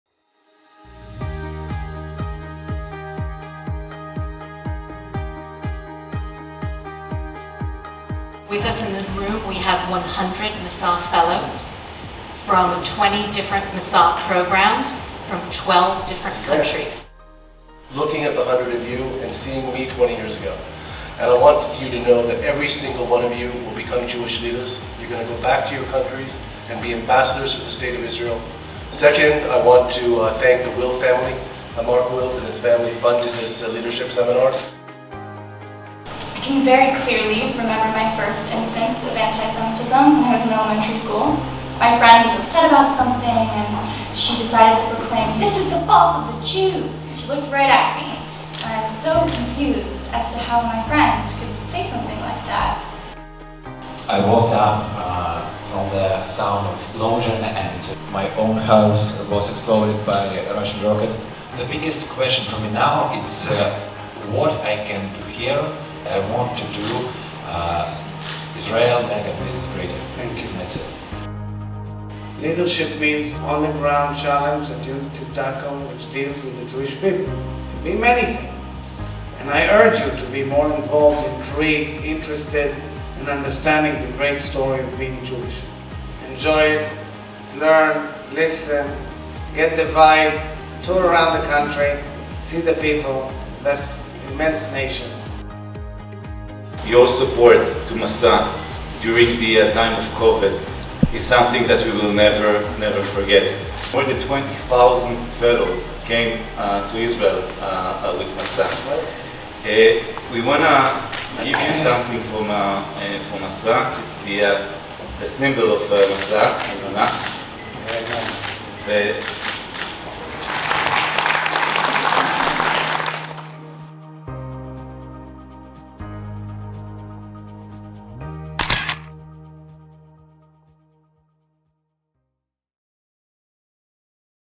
Masa Fellows visiting the President's Residence and meeting Isaac Herzog, president of Israel
During the Masa Wilf Leadership Summit, our Fellows had the chance to meet the President of Israel, Isaac Herzog, while having a conversation about leadership, different personal challenges, and the future of the Jewish world and Israel.